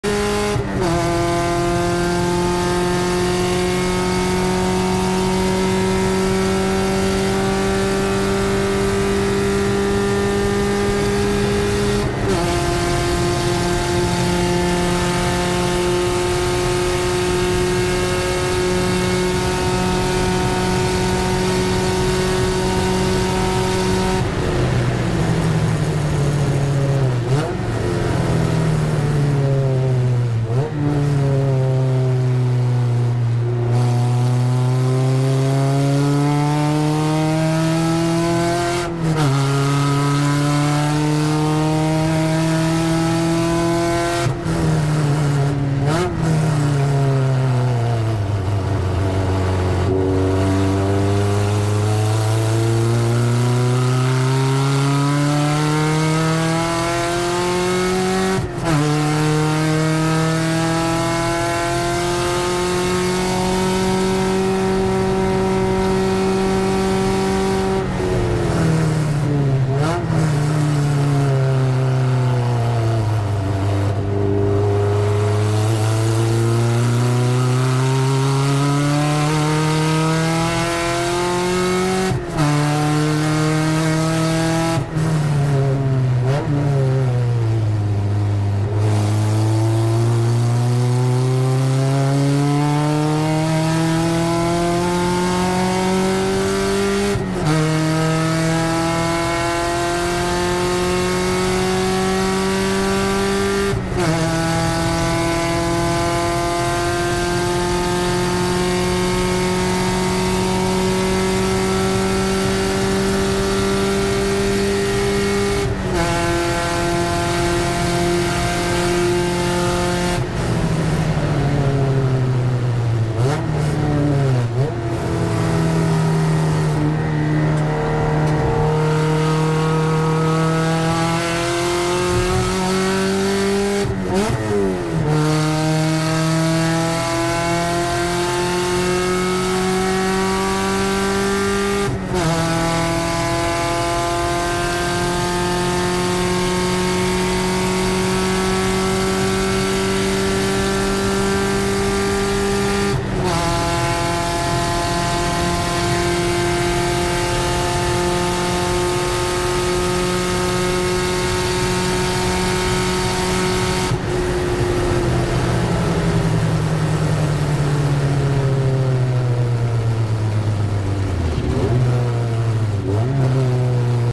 My 1st SCCA race/leaning Daytona ...June 2024
My first ever SCCA (novice) race in a friends 968 Porsche.
So much to learn., but a blast to drive and learn this cool track.
Hard to see in vid, but had to avoid my exhaust (fell off early in race) as it was laying between corners 3 and 4.